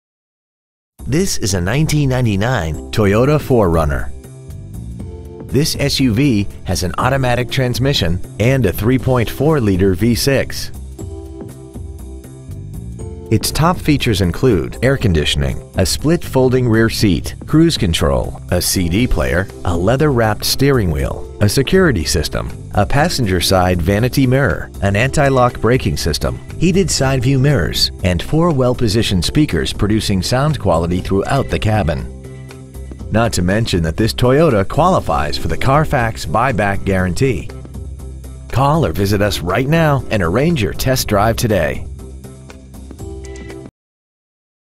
Toyota 4runner Tulsa OK
Year: 1999
 Make: Toyota
 Model: 4runner
 Engine: V6 3.4 Liter
 Trans.: Automatic
 Exterior: Silver
 Miles: 177,307
 Interior: Gray

 6868 East B.A. Frontage Rd.

 Used 1999 Toyota 4runner Tulsa OK